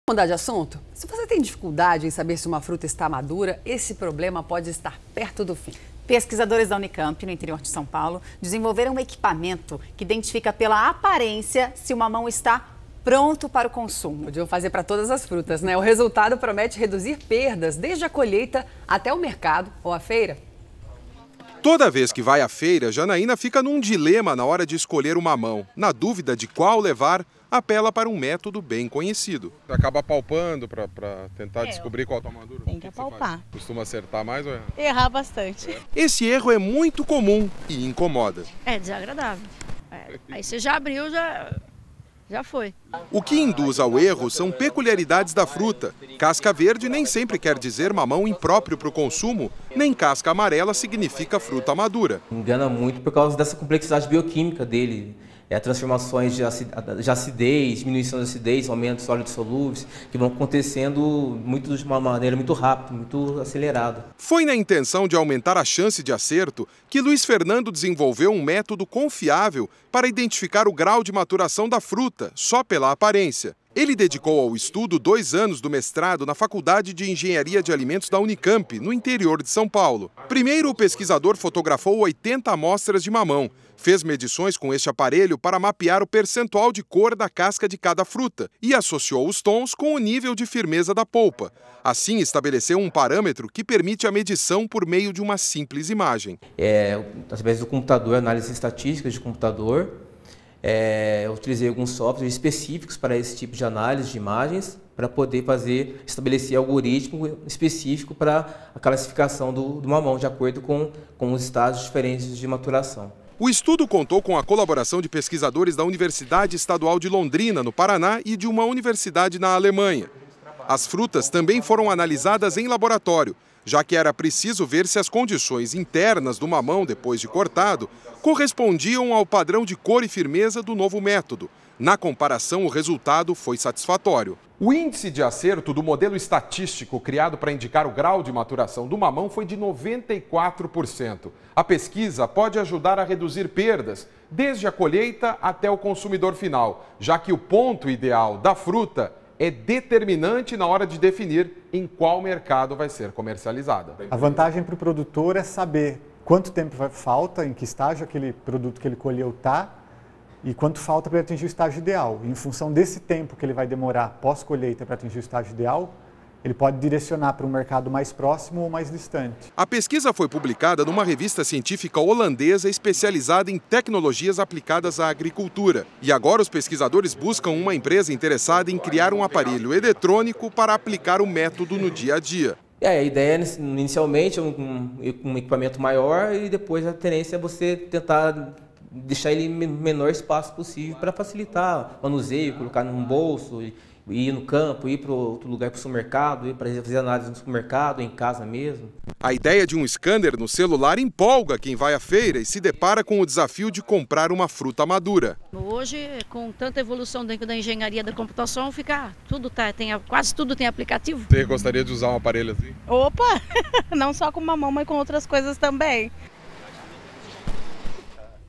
Pesquisadores brasileiros desenvolvem equipamento que identifica se fruta está madura
O equipamento foi criado por um aluno do mestrado em engenharia de alimentos da UNICAMP, universidade no interior de São Paulo, e promete reduzir desperdícios desde a colheita até o mercado.